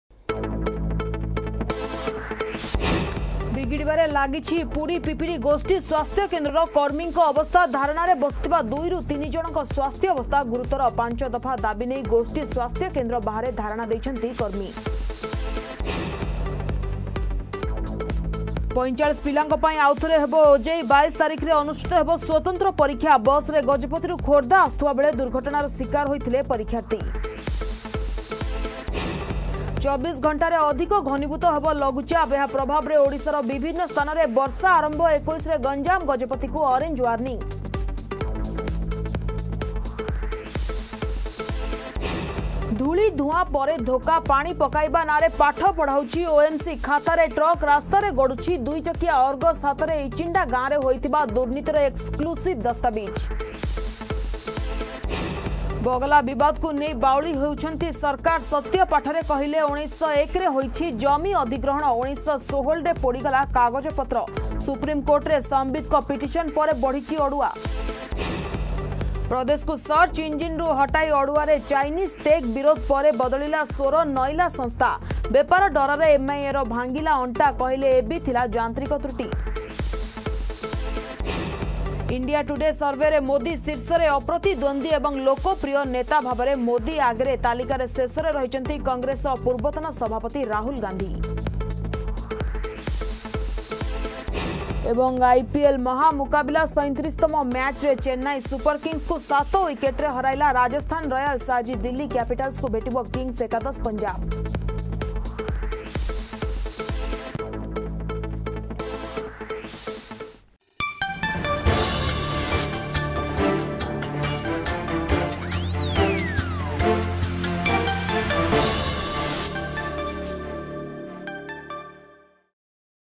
Headlines 8am
ବିଗିଡ଼ିବାରେ ଲାଗିଛି ପୁରୀ ପିପିଲି ଗୋଷ୍ଠୀ ସ୍ୱାସ୍ଥ୍ୟକେନ୍ଦ୍ରର କର୍ମୀଙ୍କ ଅବସ୍ଥା

45 ପିଲାଙ୍କ ପାଇଁ ଆଉ ଥରେ ହେବ OJEE

24 ଘଣ୍ଟାରେ ଅଧିକ ଘନୀଭୂତ ହେବ ଲଘୁଚାପ

ଧୂଳି, ଧୂଆଁ ପରେ ଧୋକା

ବଗଲା ବିବାଦକୁ ନେଇ ବାଉଳି ହେଉଛନ୍ତି ସରକାର

ପ୍ରଦେଶକୁ ସର୍ଚ୍ଚ ଇଂଜିନରୁ ହଟାଇ ଅଡୁଆରେ ଚାଇନିଜ୍ ଟେକ୍ .

ଇଣ୍ଡିଆ ଟୁଡେ ସର୍ଭେରେ ମୋଦି ଶୀର୍ଷରେ

ଆଇପିଏଲ ମହାମୁକାବିଲା